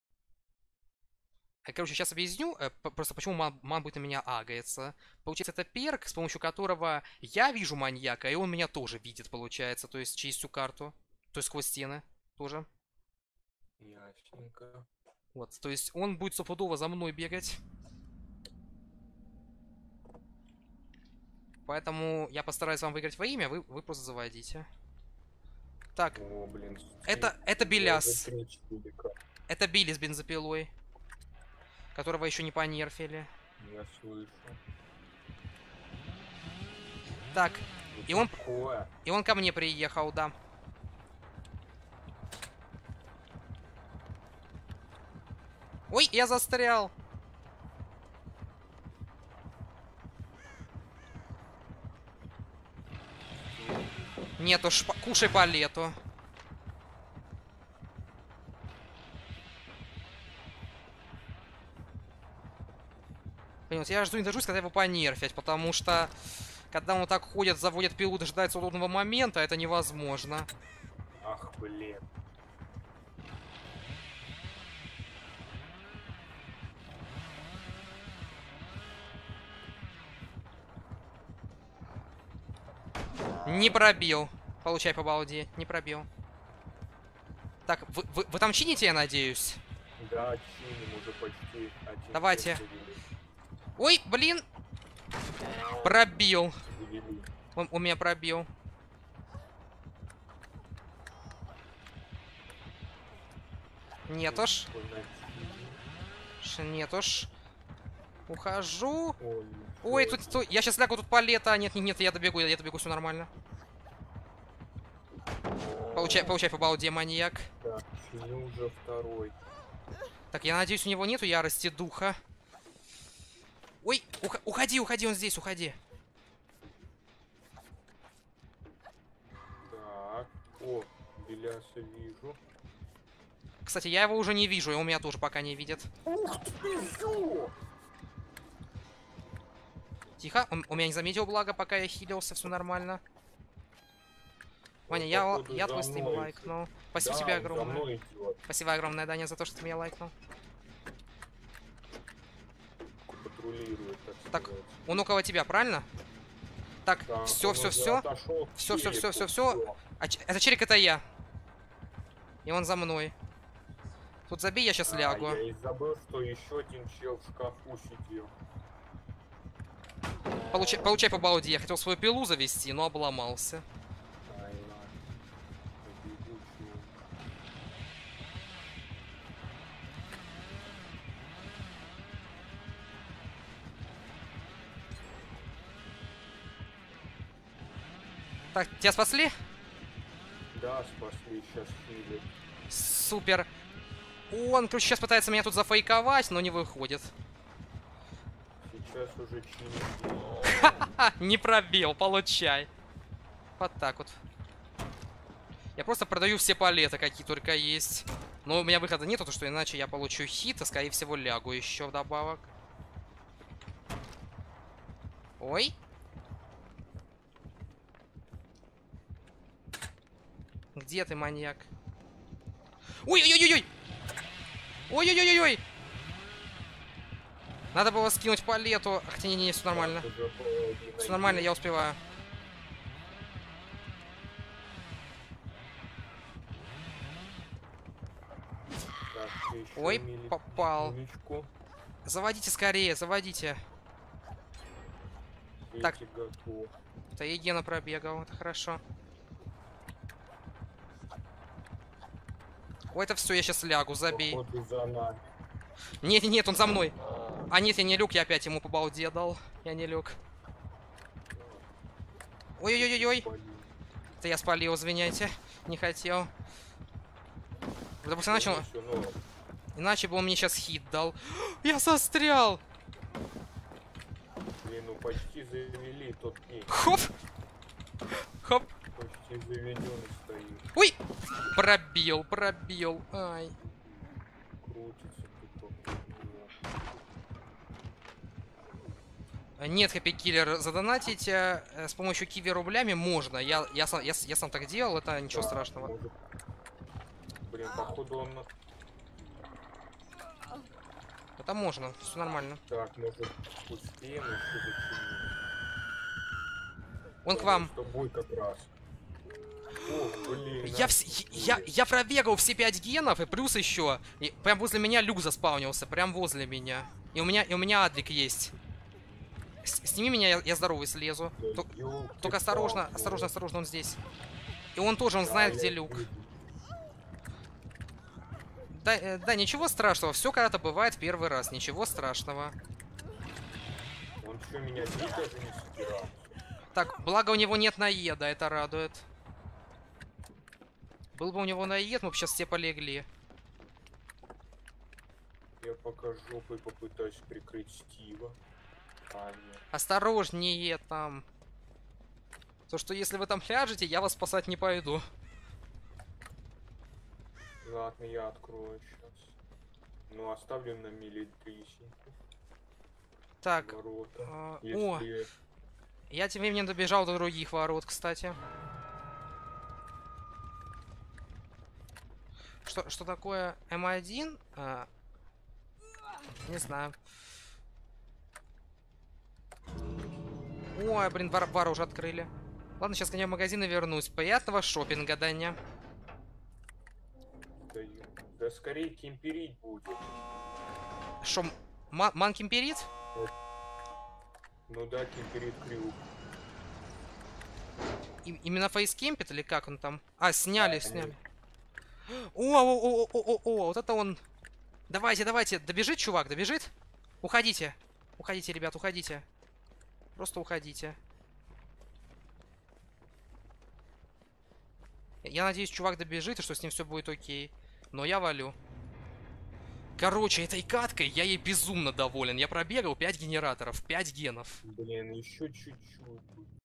Отмасил Старого Хилли-Билли 5 Генов!!
Момент с моего относительно старого стрима, где я решил взять глазик и побегать от манов.